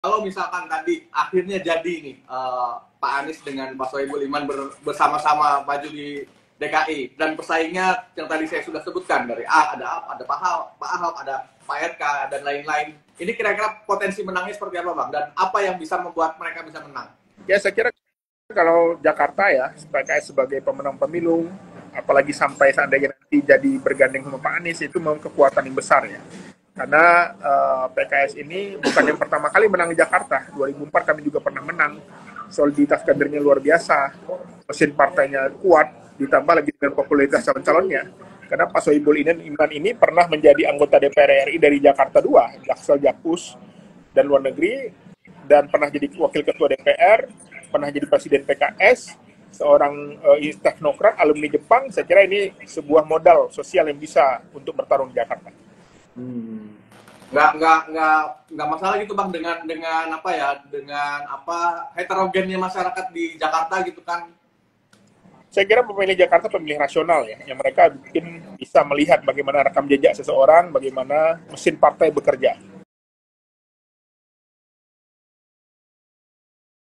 PKS OPTIMIS ANIES DAN SOHIBUL IMANMAMPU RAIH DUKUNGAN DI PILGUB DKI
PKS Optimis Anies dan Sohibul Iman Mampu Raih Dukungan di Pilgub DKI

Jika Anies Baswedan dan Sohibul Iman maju bersama dalam Pemilihan Gubernur (Pilgub) DKI Jakarta, peluang kemenangan mereka dinilai cukup besar. Dalam pemilihan ini, mereka akan bersaing dengan calon-calon kuat seperti Basuki Tjahaja Purnama (Ahok) dan Ridwan Kamil (RK).

"Seandainya nanti jadi bergandeng dengan Pak Anies, itu memang kekuatan yang besar. PKS bukan yang pertama kali menang di Jakarta, pada 2004 kami juga pernah menang," jelas Juru Bicara PKS Muhammad Iqbal

Dengan kombinasi ini, pasangan Anies dan Sohibul Iman diharapkan mampu menarik dukungan luas dari masyarakat Jakarta dan memperkuat posisi PKS dalam koalisi untuk Pilgub mendatang.